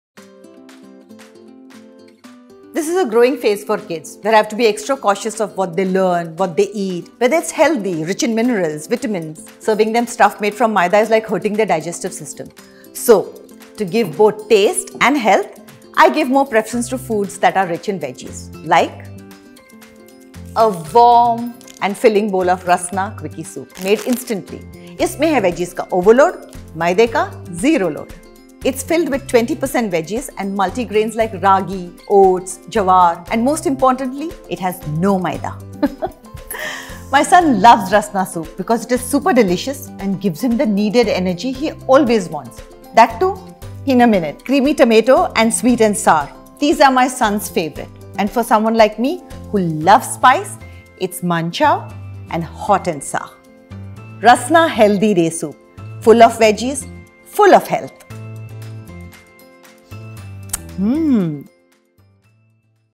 Rioconn X Rasna | Here comes the HEALTHY!
Rasna recently roped the most beautiful Sonali Bendre for their all-new Healthy Day Range.

Rioconn curated the whole process including the idea to launch the Healthy Day Range. The ad campaign is now live across multiple mediums and channels on the internet.

Our Challenge: To match the frequency & justify the collaboration with Sonali Bendre while maintaining the brand image of both- the actress and Rasna.
Sonali is an independent identity known for her bold choices, acting skills, beauty and her way to live a healthy life, and so, she was our go-to choice when it came to promotion of the Healthy Soups.

Do let us know if we did a good job.